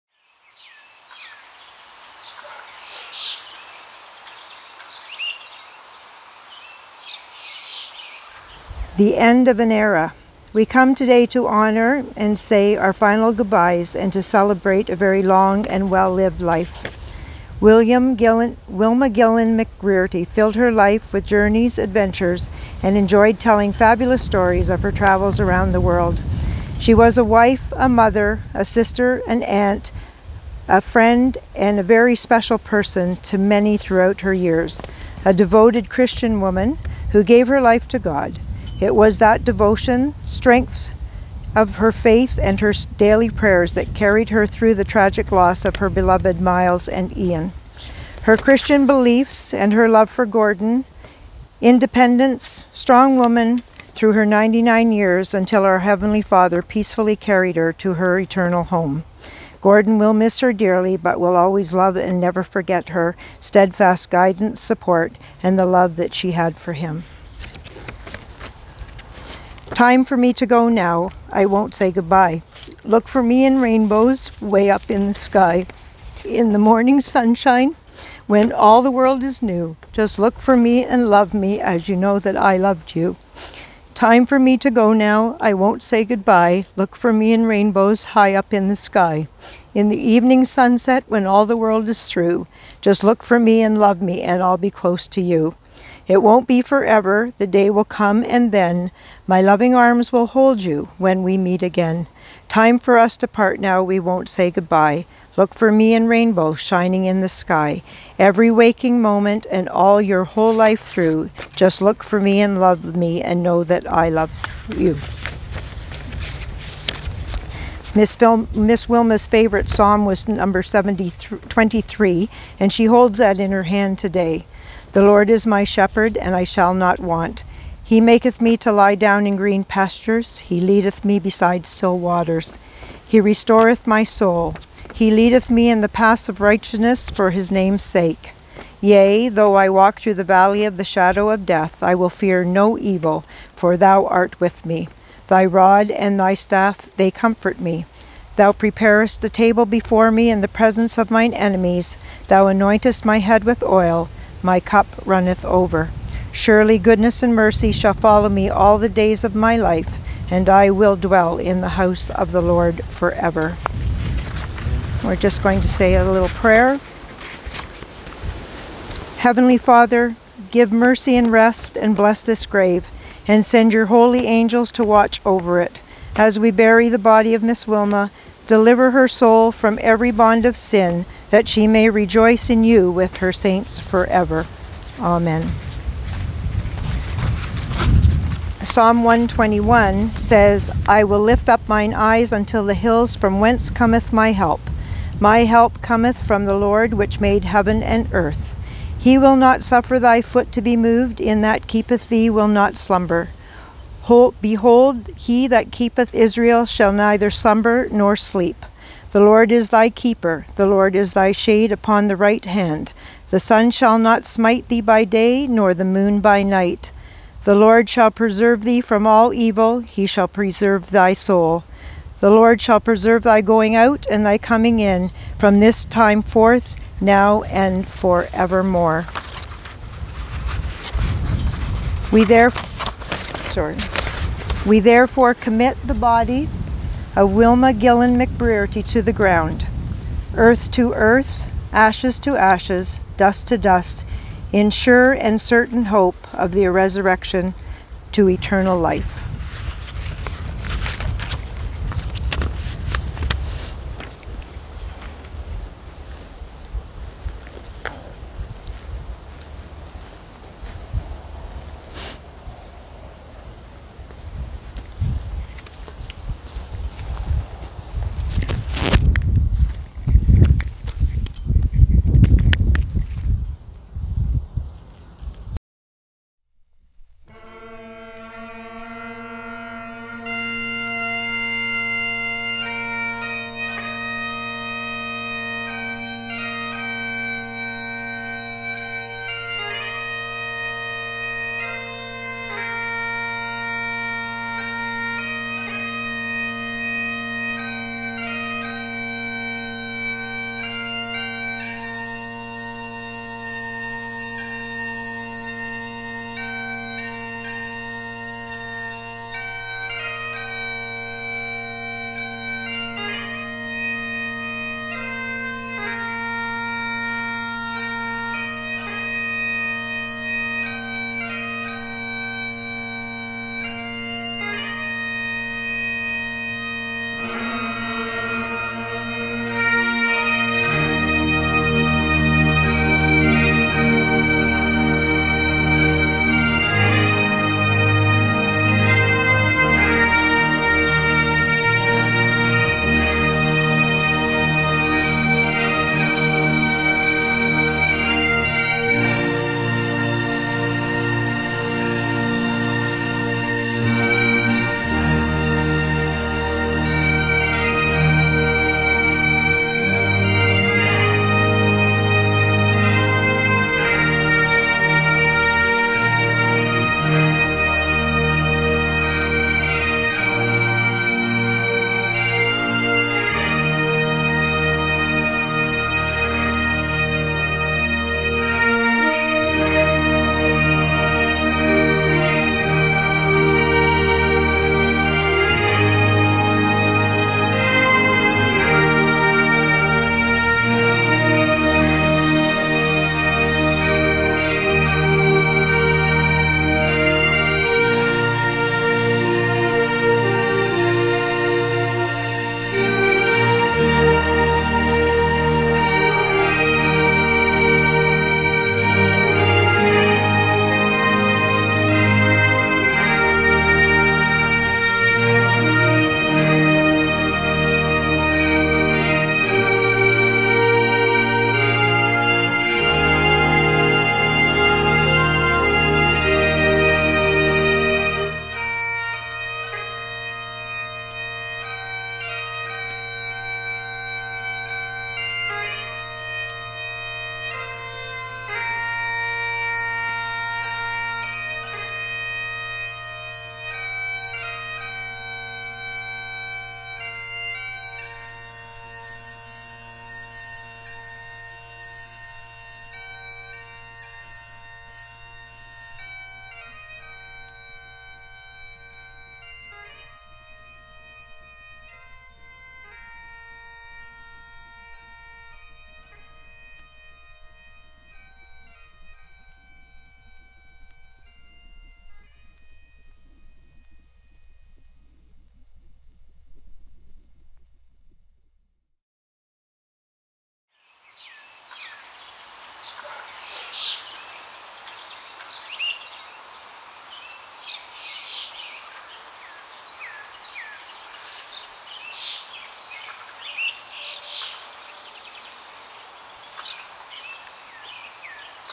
WilmaFuneral
Thursday, December 8th, we laid to rest my Little Scottish Mother, two months shy of her 100th birthday. She was flown down from Canada then buried beside my father, Myles, in a small, rustic cemetery in La Feria, Texas.

She lived an amazing life, from sleeping in London's Underground to avoid the bombs of WWII, leaving her native Scotland to come to a small northern mining town with 8-month winters, retiring early with my father to travel, including backpacking around the world.

She will be missed.